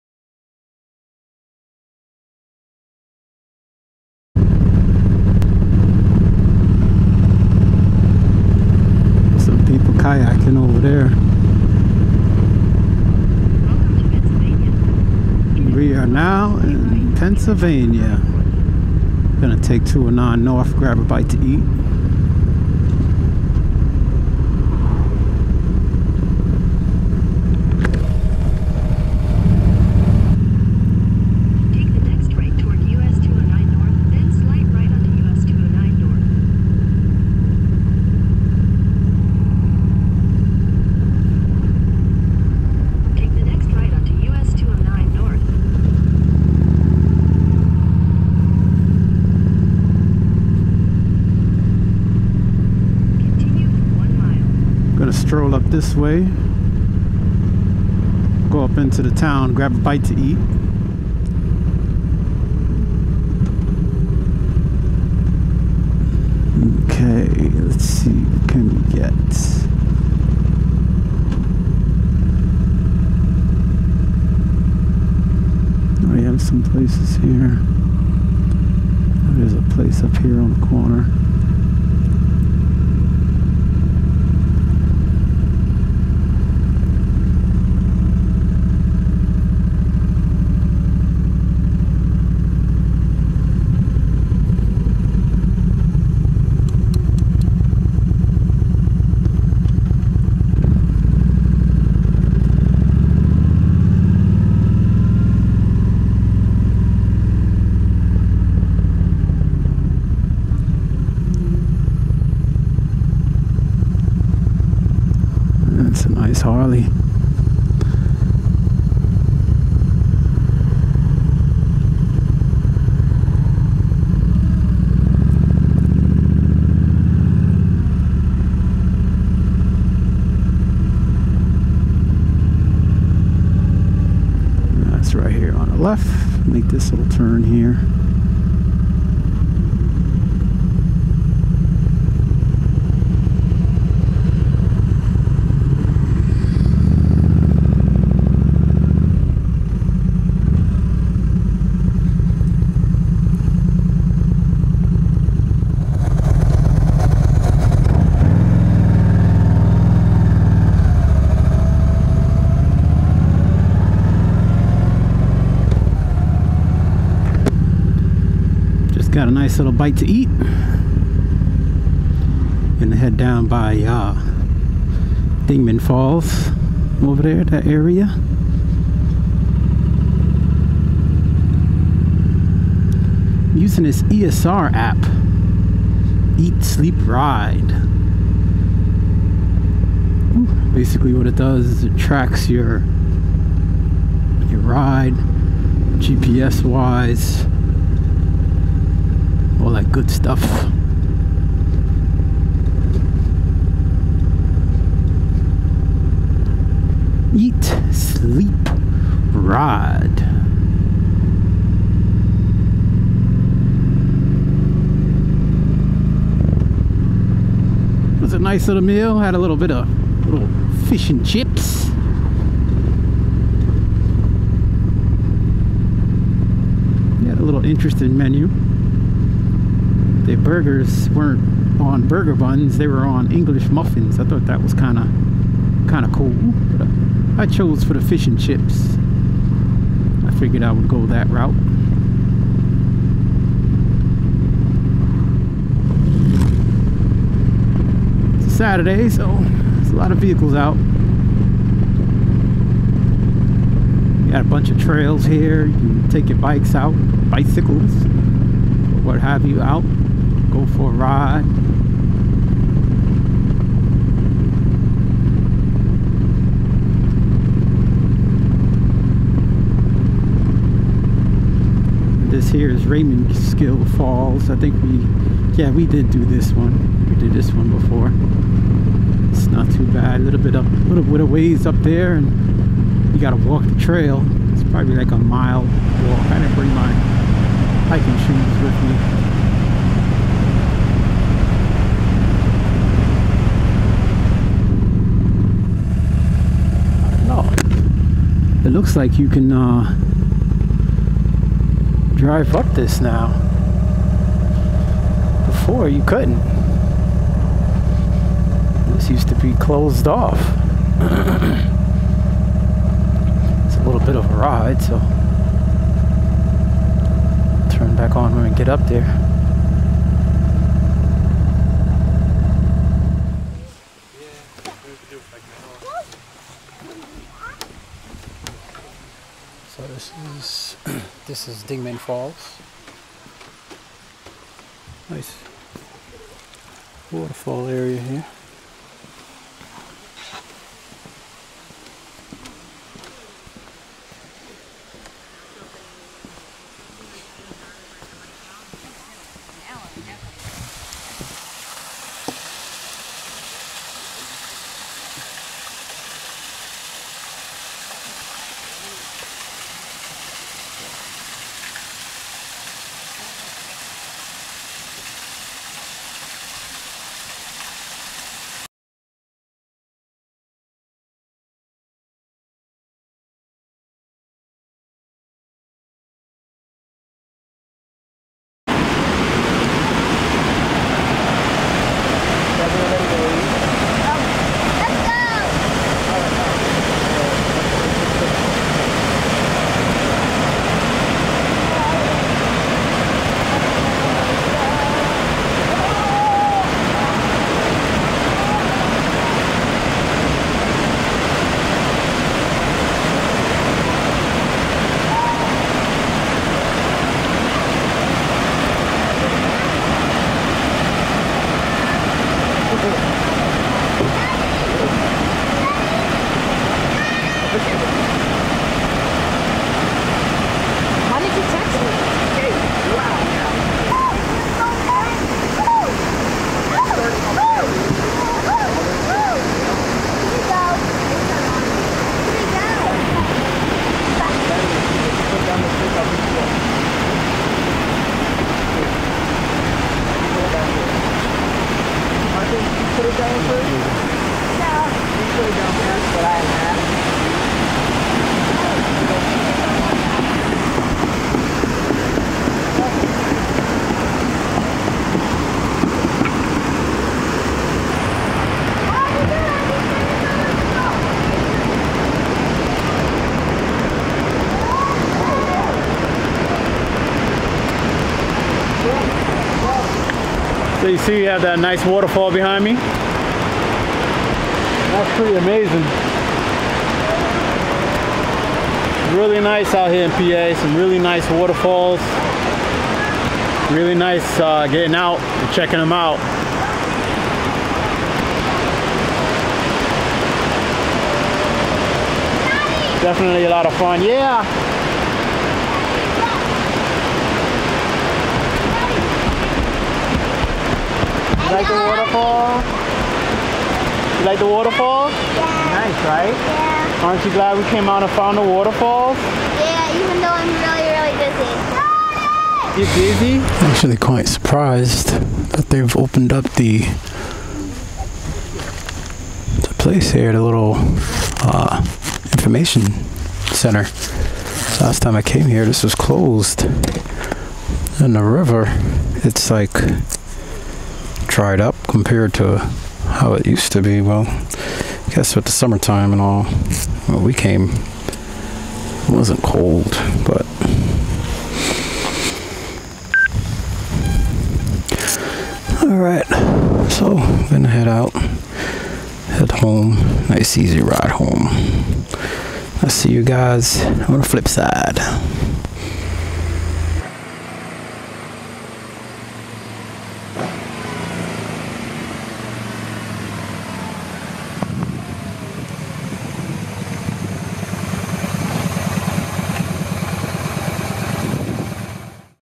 Venture to Dingman Falls | part 2 | Vlog#184
Part 2 of my Venture ride to Dingman Falls. We grab a bite to eat , then head down towards the falls.

Cameras
Sony - fdrx3000 (used here)
Xioami Yi 4k
Xioami Yi